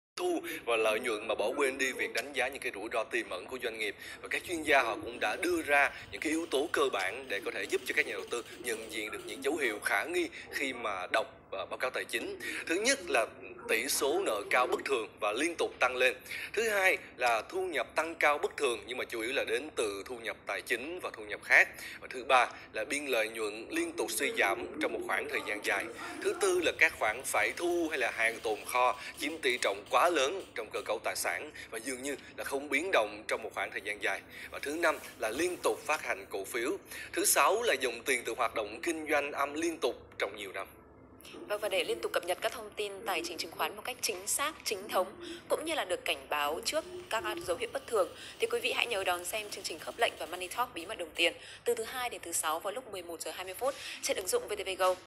Nhận diện doanh nghiệp xào nấu quá báo cáo tài chính
thông tin rất quan trọng.nhưng quan trọng hơn là thông tin đúng, và công cụ phân tích chuẩn.biết thị trường đi về đâu.đến đâu, xác xuất thăng phải cao hơn xác xuất thua thì ta mới vào lệnh, mua bán cổ phiếu.